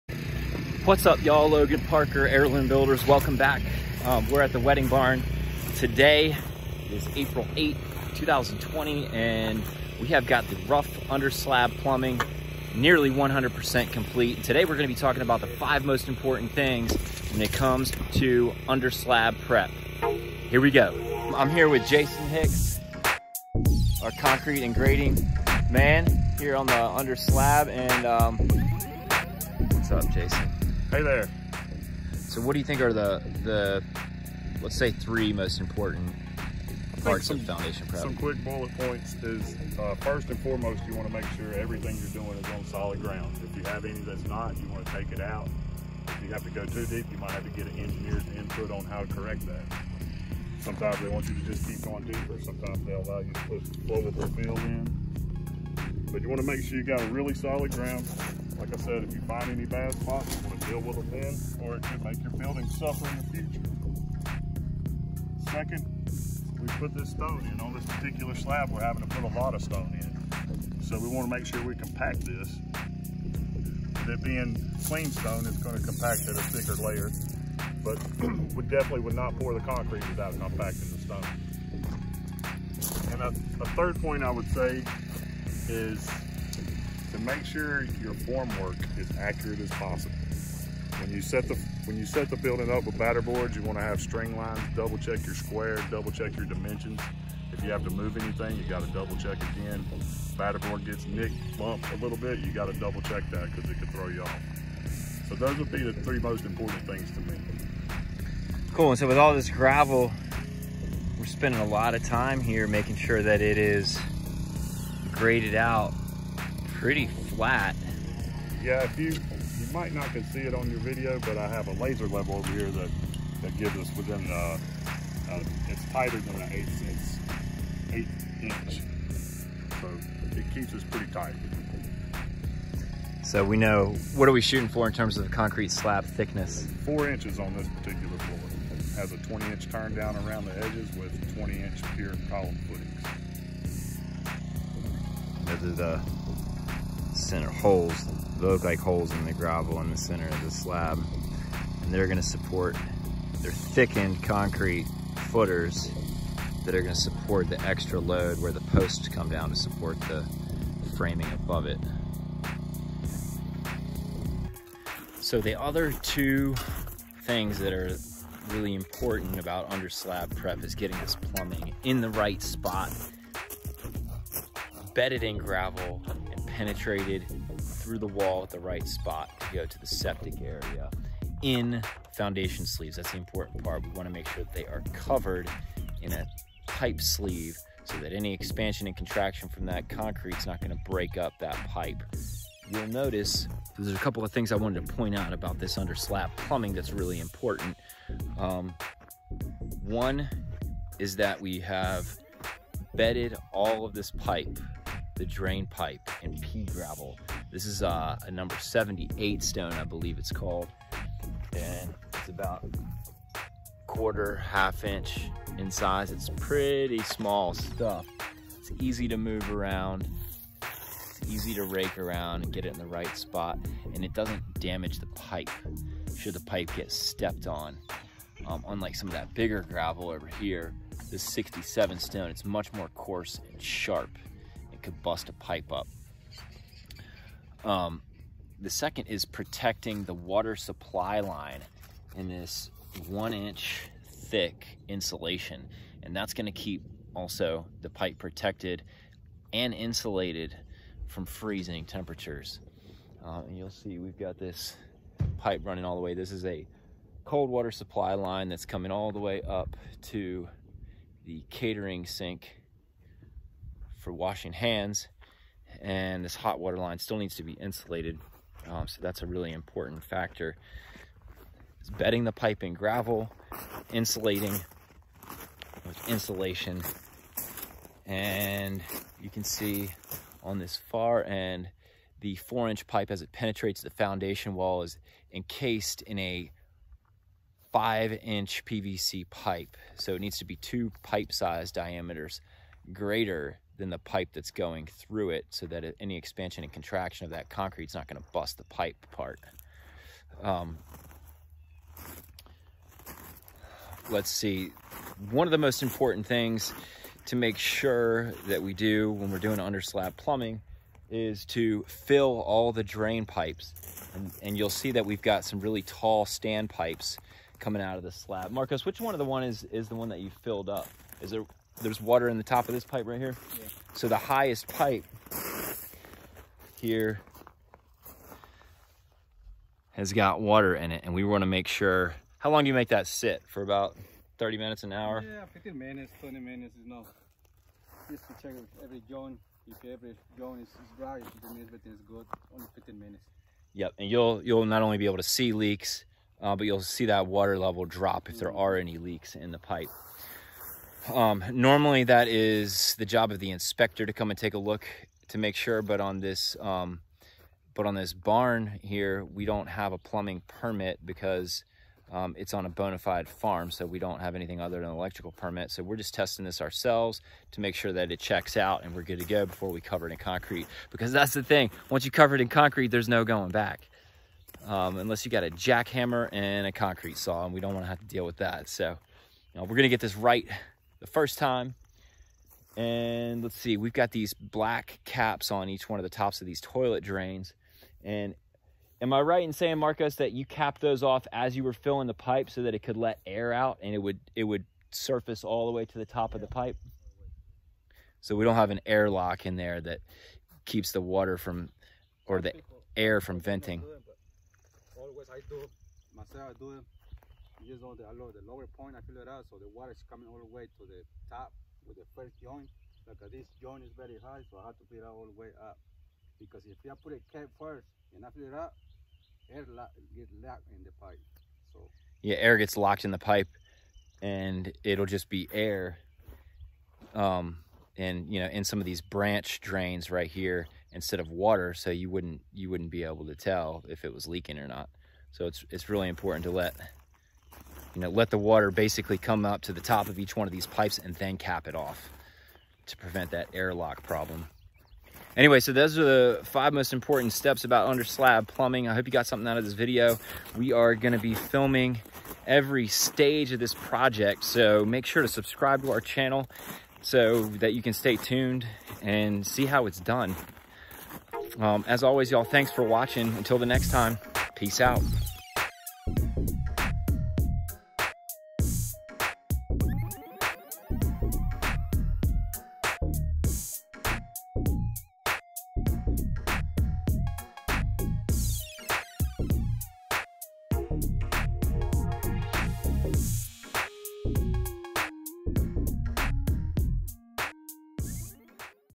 Underslab Prep - 5 Tips for a solid foundation and water-tight plumbing
We are building a monolithic slab foundation for an 8000 sq ft wedding venue with plumbing for 6 toilets, 4 vanity sinks, and a catering sink for washing hands and food prep. We talk about 5 of the best practices for getting a solid concrete foundation and water tight plumbing.